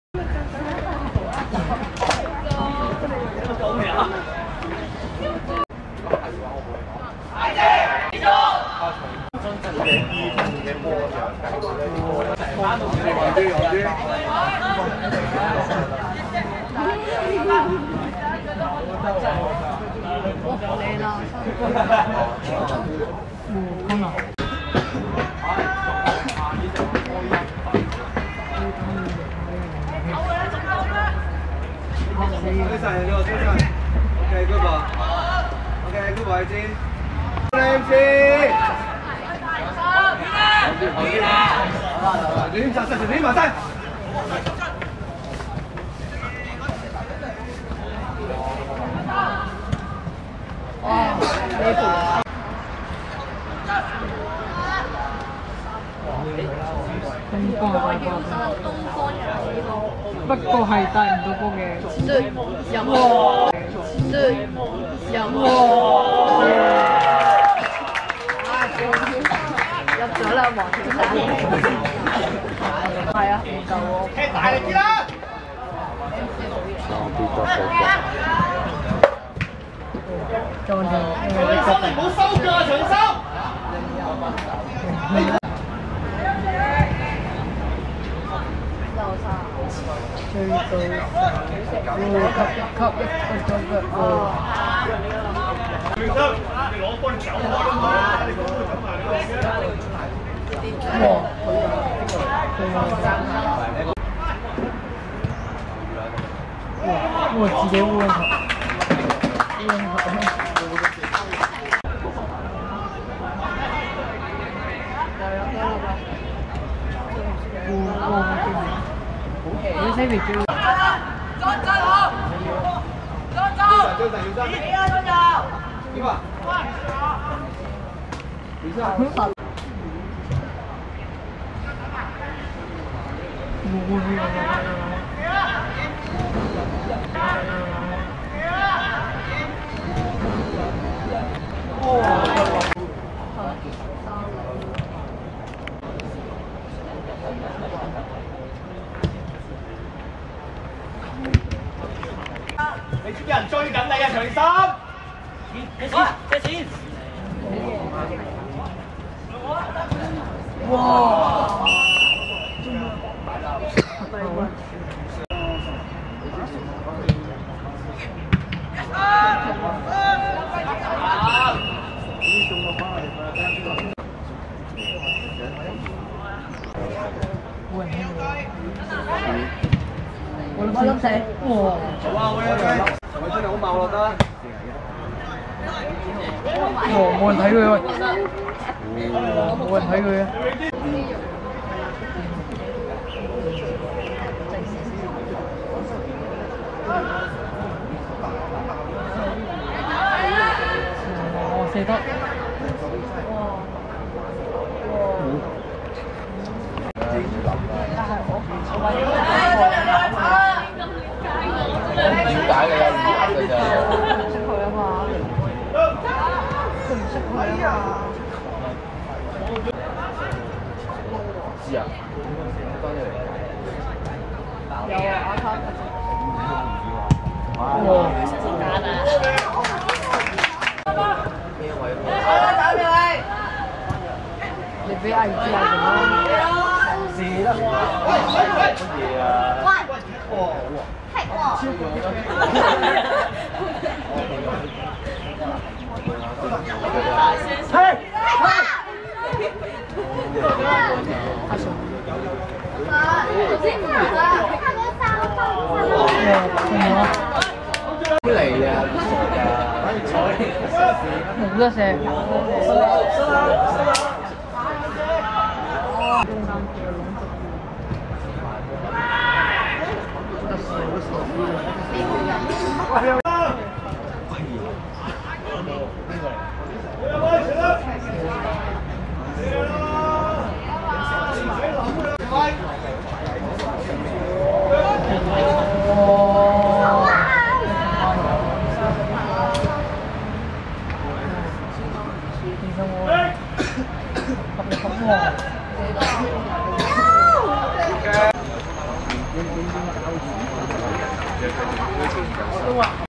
天官vs毅智(2016.2.22.元朗學界足球乙組)精華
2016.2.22.天業路仿真草塲 15:10 天官(綠) 0 : 3 毅智(藍)